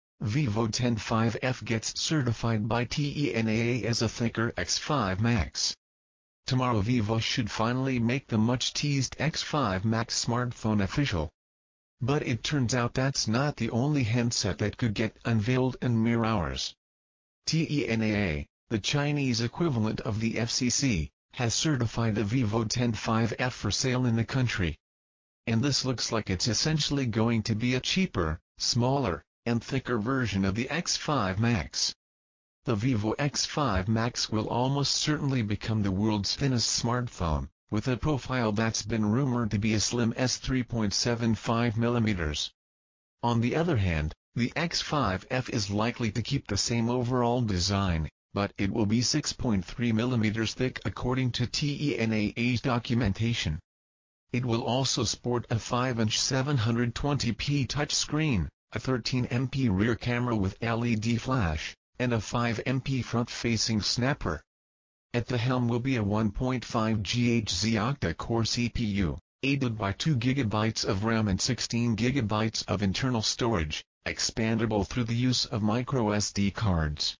Vivo X5F gets certified by TENAA as a thicker X5 Max
Tomorrow Vivo should finally make the much-teased X5 Max smartphone official. But it turns out that's not the only handset that could get unveiled in mere hours.
TENAA, the Chinese equivalent of the FCC, has certified a Vivo X5F for sale in the country. And this looks like it's essentially going to be a cheaper, smaller, and thicker version of the X5 Max.

The Vivo X5 Max will almost certainly become the world's thinnest smartphone, with a profile that's been rumored to be as slim as 3.75mm.
On the other hand, the X5F is likely to keep the same overall design, but it will be 6.3mm thick according to TENAA's documentation. It will also sport a 5-inch 720p touchscreen, a 13 MP rear camera with LED flash, and a 5 MP front-facing snapper.
At the helm will be a 1.5 GHz octa-core CPU, aided by 2GB of RAM and 16GB of internal storage, expandable through the use of microSD cards.